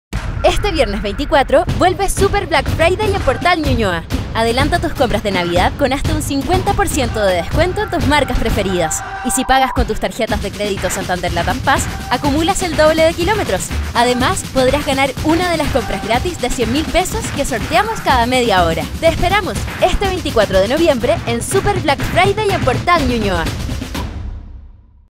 Súper Black Friday Portal Ñuñoa 2017
Llega el Súper Black Friday a Portal Nuñoa, participa por una de las compras gratis de $100.000 pagando con tus Tarjetas de Crédito Santander Latam Pass y además acumula el doble de Kms. ¡Te esperamos este 24 de noviembre con hasta un 50% de dcto. en tus marcas preferidas! Adelanta tus compras de navidad.✨